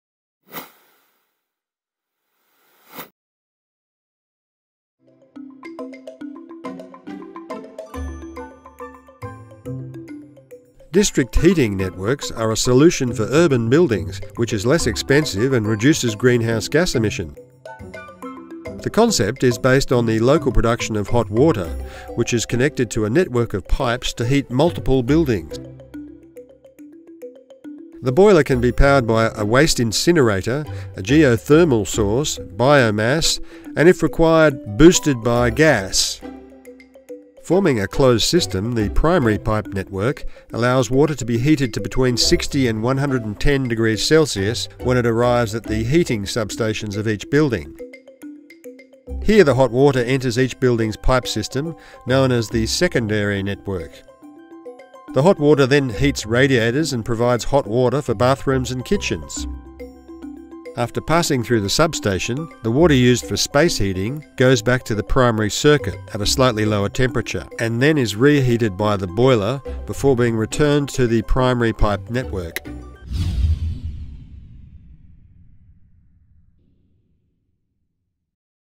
Heating Network
District heating networks are a solution for urban buildings which are less expensive and reduce greenhouse gas emissions.